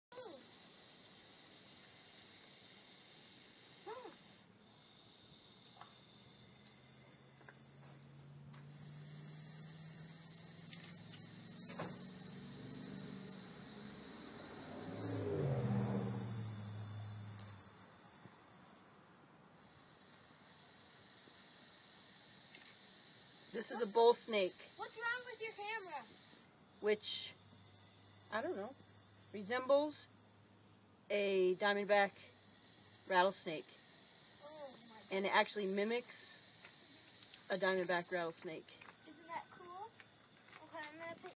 Bull snake Greeley, co
I stumbled upon this snake while cleaning up some old plastic that had settled near the side of the garage.  He is about 5 feet long and too heavy to pick up with a stick-- I tried!